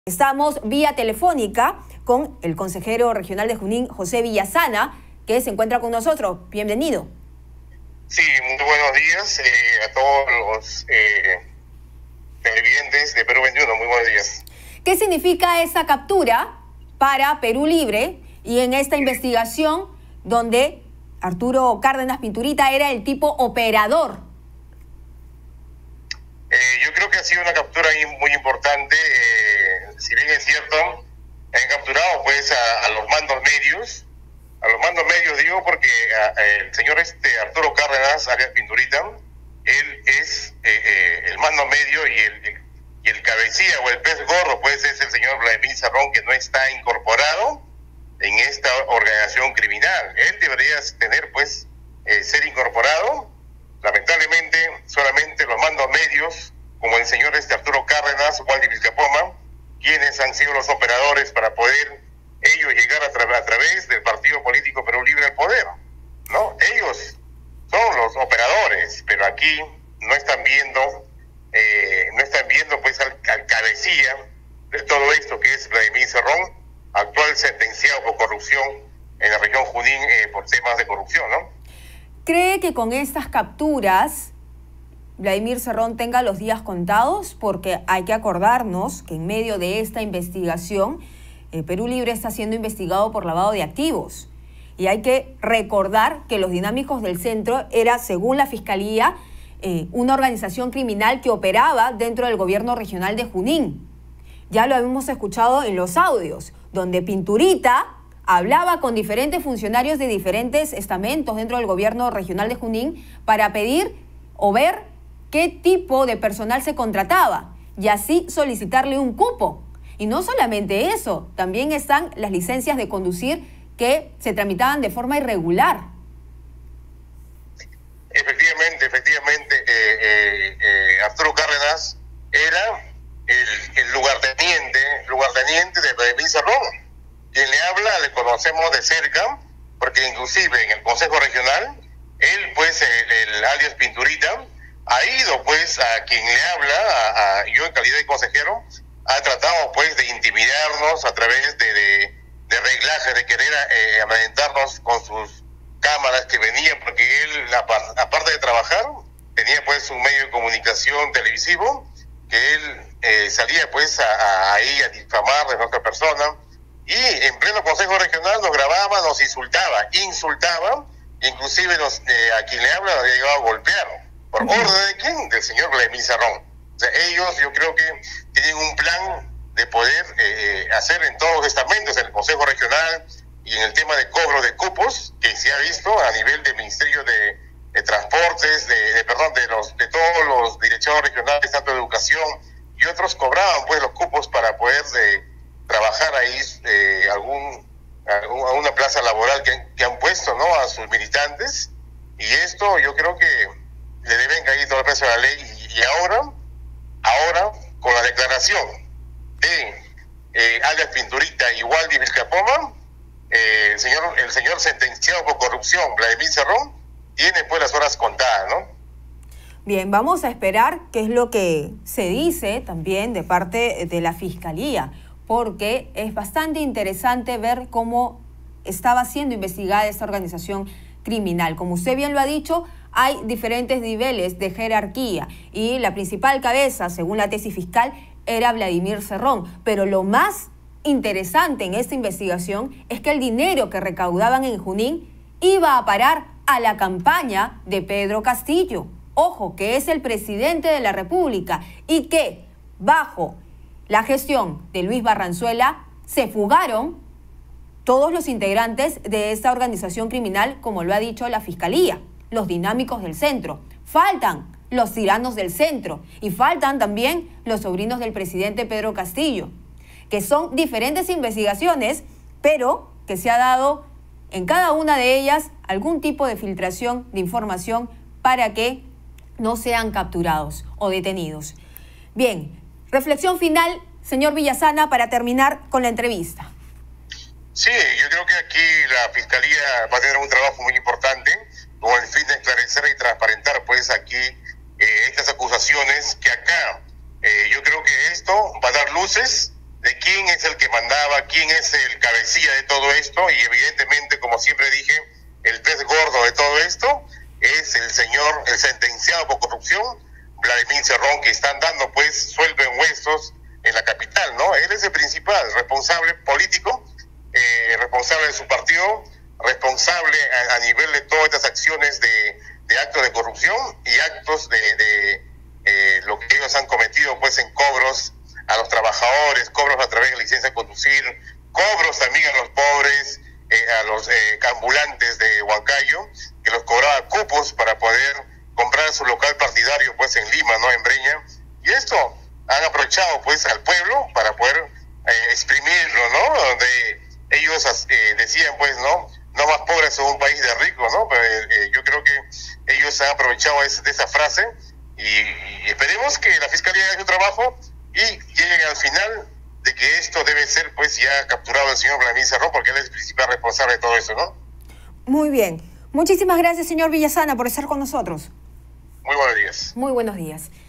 José Villazana sobre la captura de 'Pinturita': "Cerrón tiene las horas contadas"
Perú21TV conversó con José Villazana consejero Regional de Junín, sobre la reciente captura de Arturo Cárdenas alias 'pinturita', mano derecha del sentenciado por corrupción Vladimir Cerrón.

Esto es LaVozDel21 en P21TV